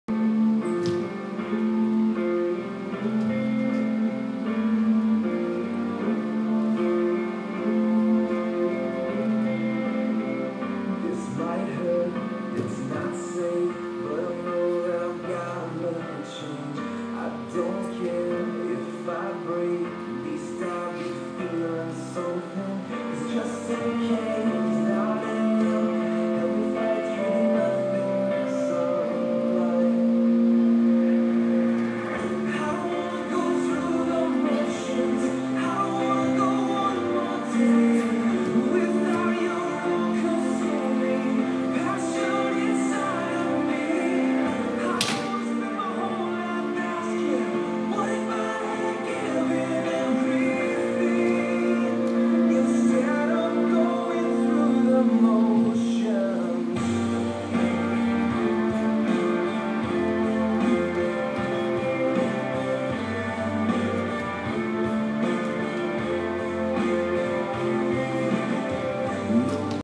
Comets Motions
Maggie and Faith performing Motions.